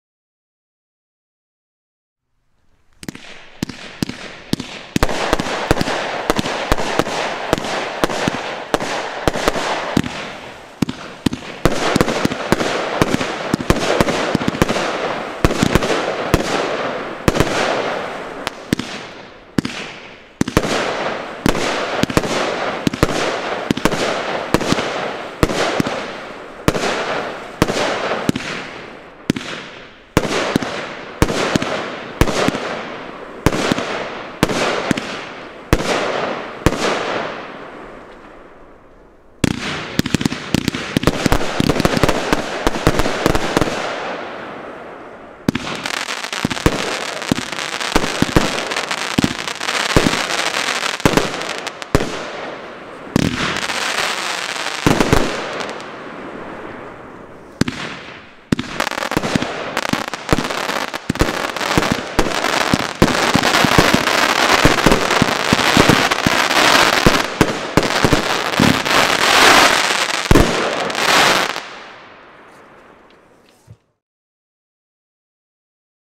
Der Hooligan - Verbundcake von Nordlicht Feuerwerk
Starker Verbund mit sehr viel Abwechslung von Nordlicht-Feuerwerk.
91 Schuss im Mischkaliber 20/25/30mm.

-gesehen und gefilmt auf einem Vorschießen von B&R-Feuerwerk Norderstedt und Feuerwerk-Postenhandel Nord Delmenhorst-